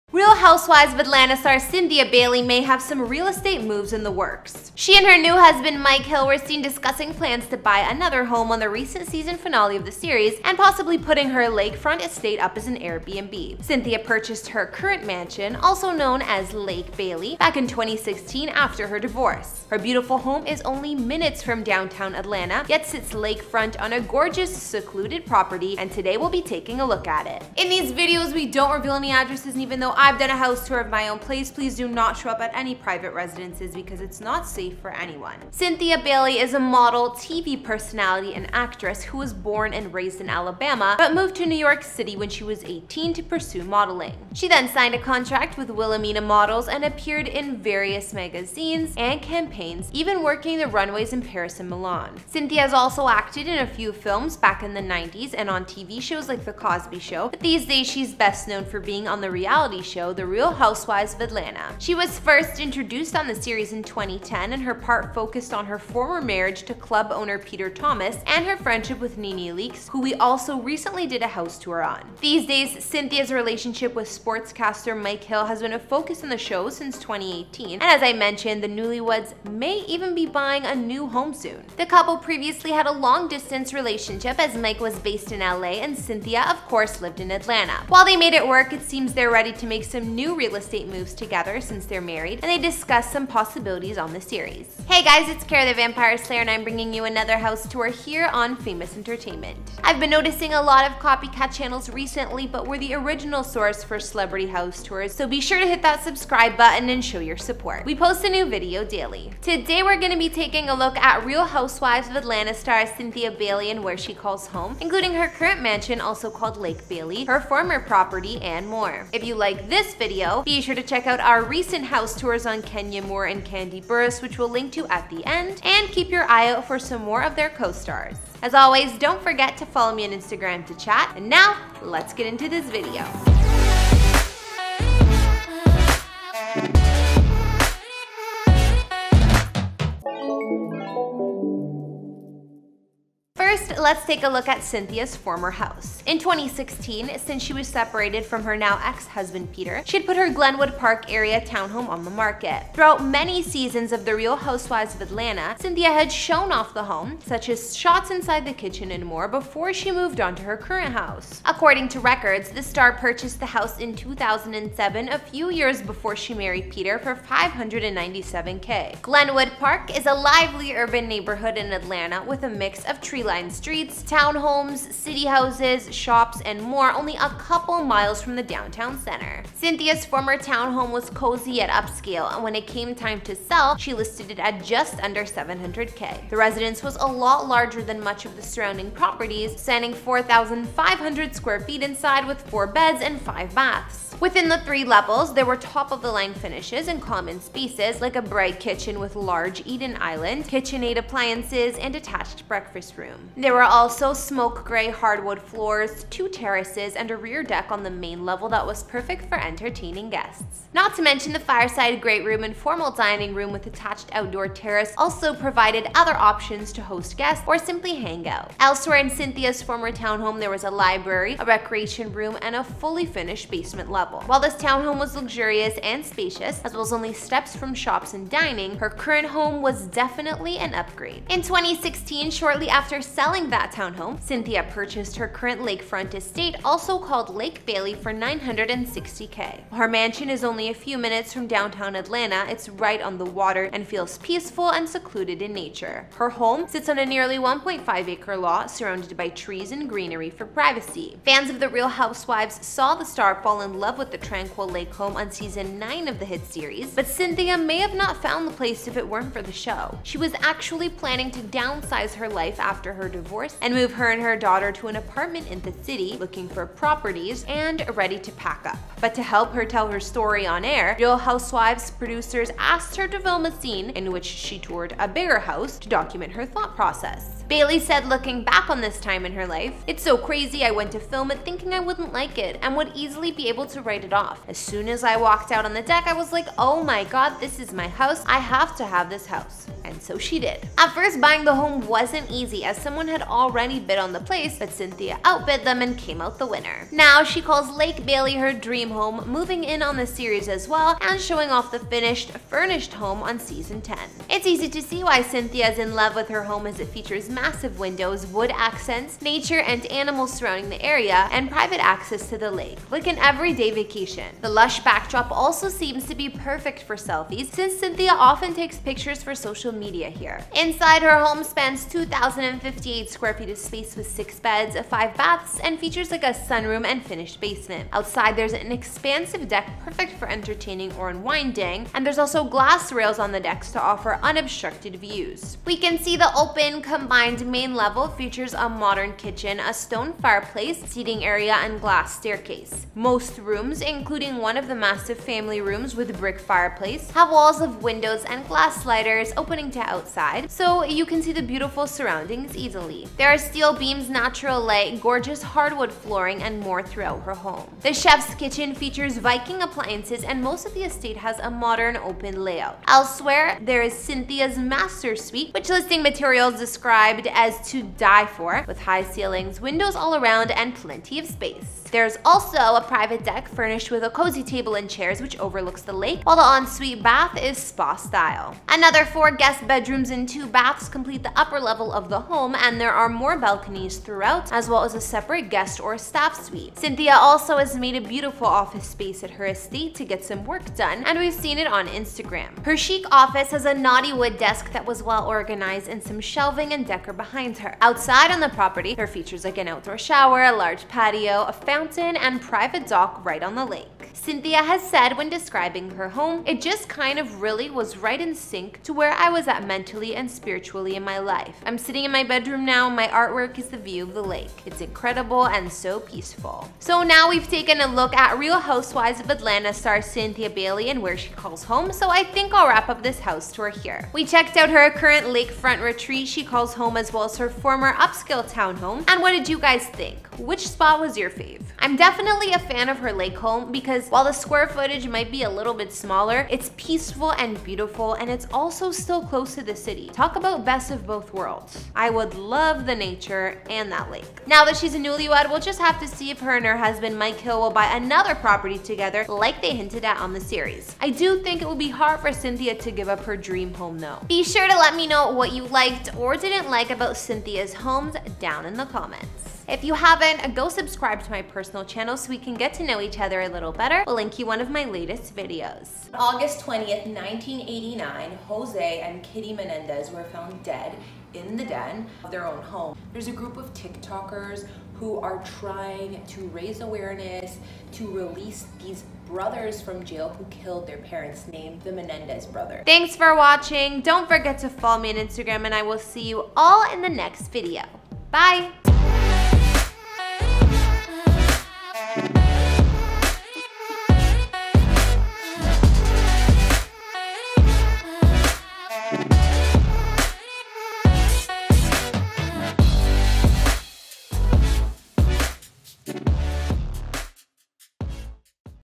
Cynthia Bailey | House Tour | Beautiful $1 Million Lakefront Mansion
Real Housewives of Atlanta star Cynthia Bailey may have some real estate moves in the works. She and her new husband Mike Hill were seen discussing plans to buy another home on the recent season finale of the series, and possibly putting her lakefront estate up as an AirbnB. Cynthia purchased her current mansion, also known as “Lake Bailey” back in 2016 after her divorce. Her beautiful home is only minutes from downtown Atlanta, yet sits lakefront on a gorgeous secluded property, and today we’ll be taking a look.